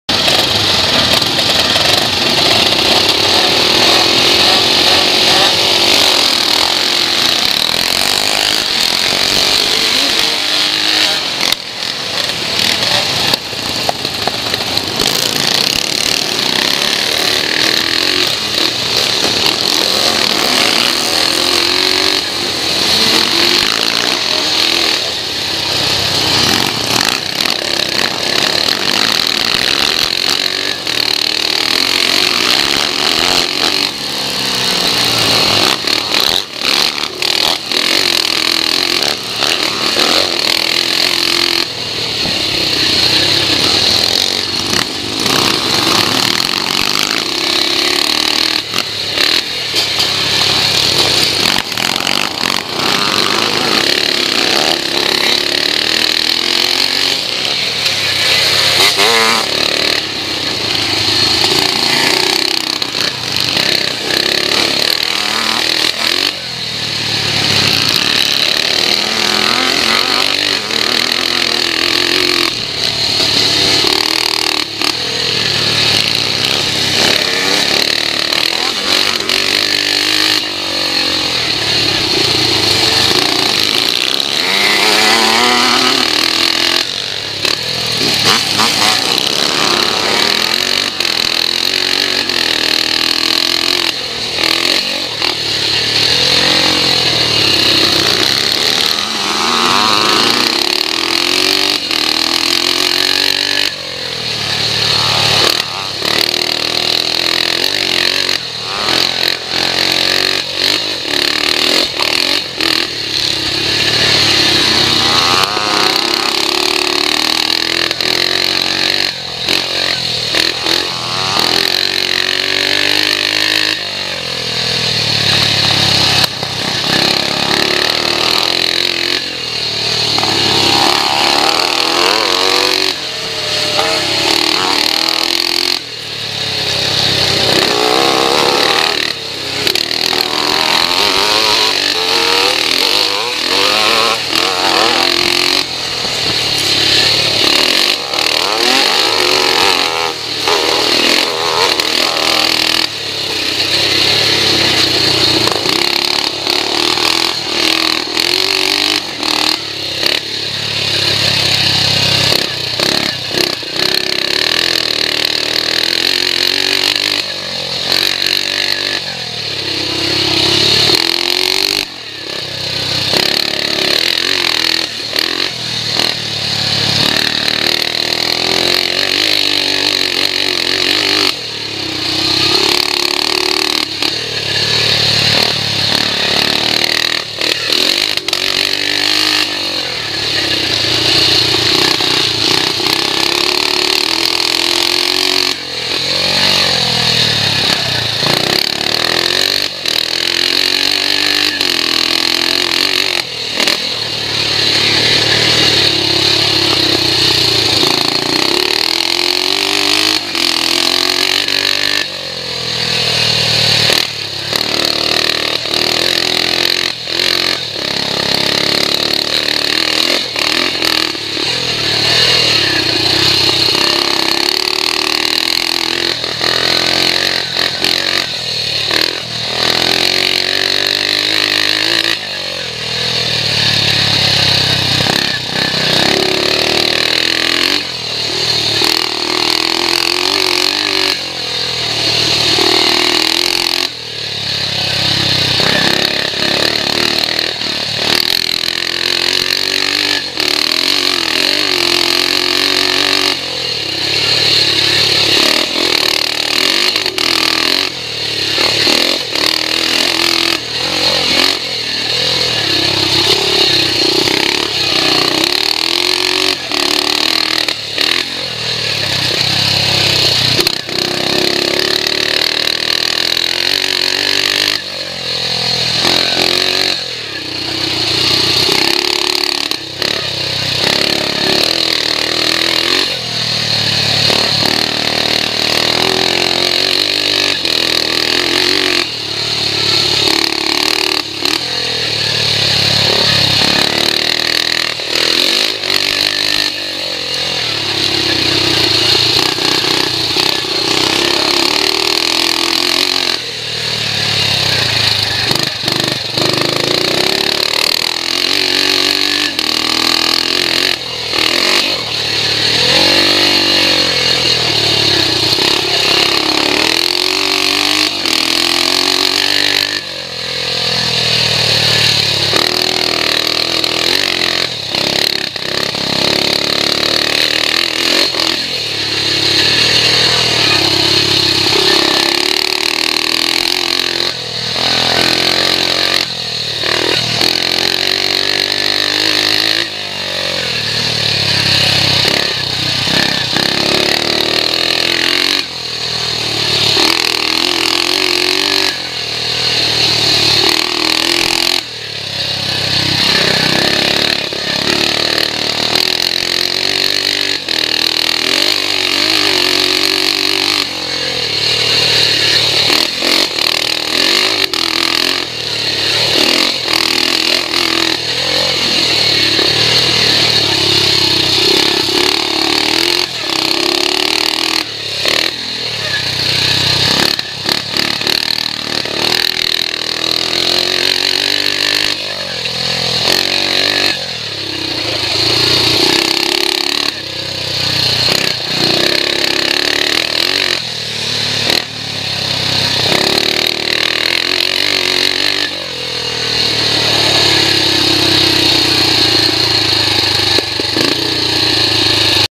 Workers Practice, Maylandsea August 2014, Pre65 MXC - 250 BSA C15 via Liquid Image 367
The Workers Practice from the August Bank Holiday meeting at Maylandsea. Took the little 250 out for a run for the first time since May and had a good battle with a few people along the way. Managed to get up to 4th.